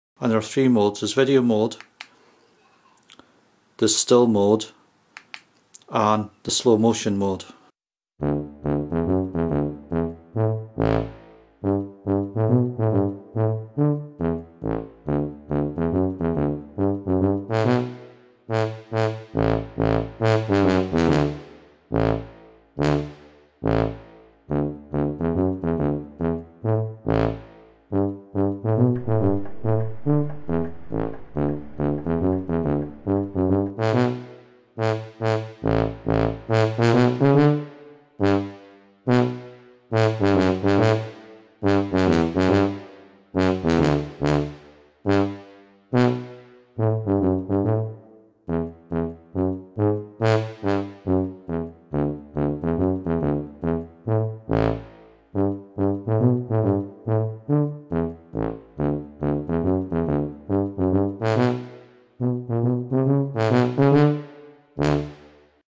Action Cam Hints & Tips: Excelvan Q8 Slow Motion Mode
A tutorial on how to use the Excelvan Q8 4K Ultra HD action camera in Slow Motion (Slo Mo) mode.  This camera has two slow motion options, 1080p @60 FPS and 720p @120 FPS.  Includes some sample clips at both speeds and real time for comparison.